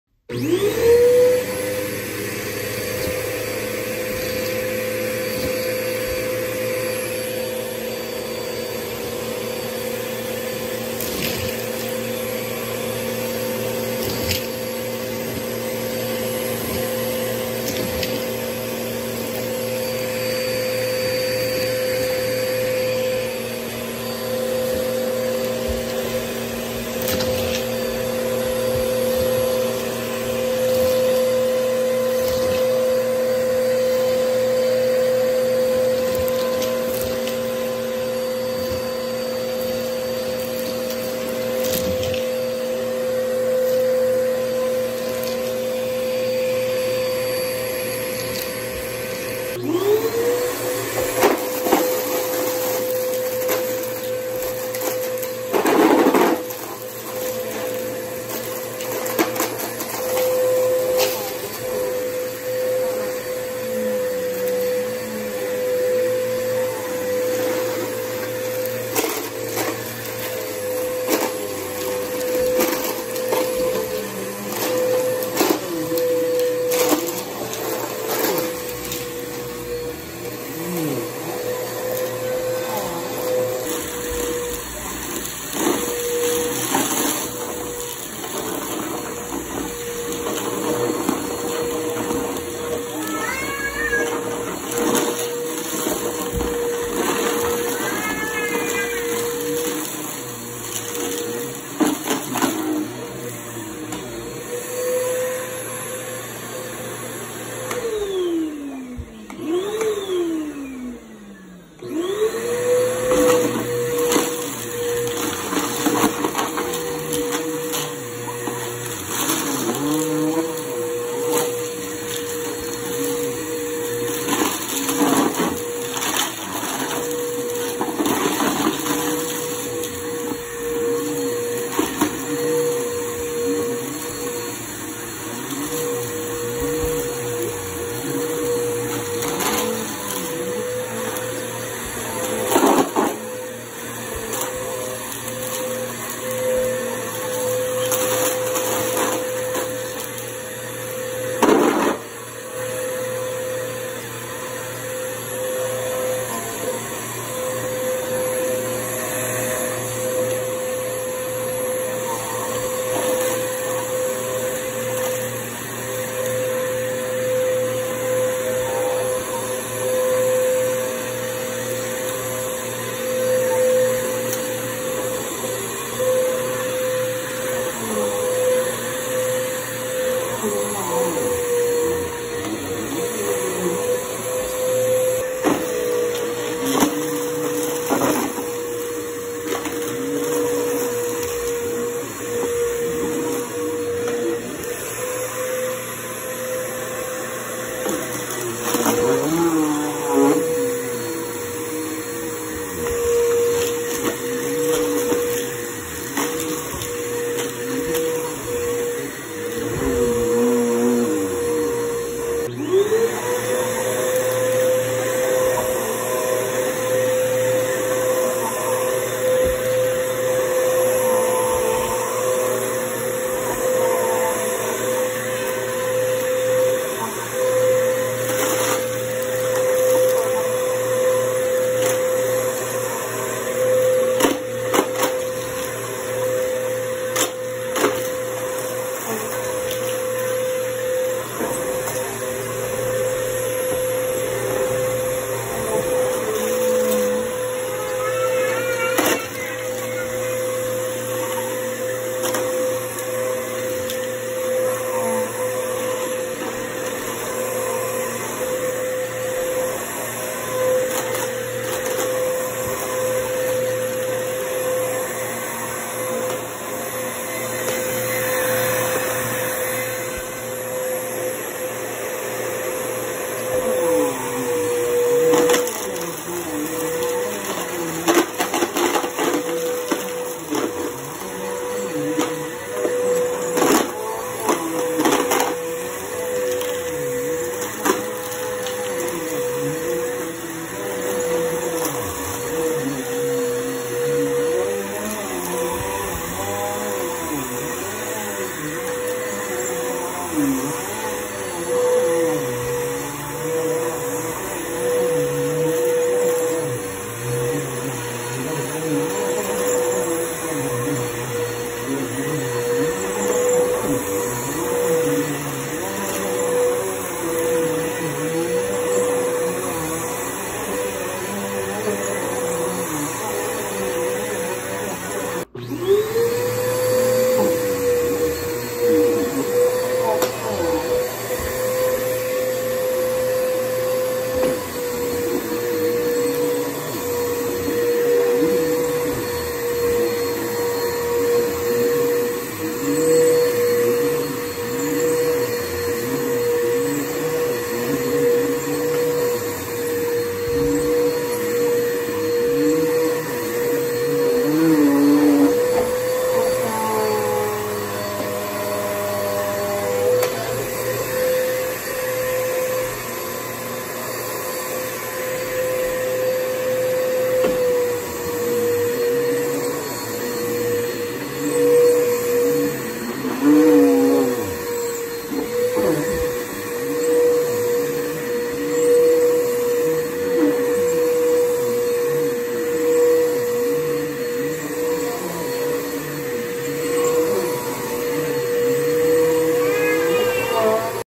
Pink rug success, video fail- dedicated to @donna loves vacs
I realized after I did the whole video that most of it recorded sideways! 🤦🏻‍♀️ sorry!!
But I’m dedicating this to @donna loves vacs because she helped me figure out how to handle this rug!! 🙌🏻 You are the best Donna!!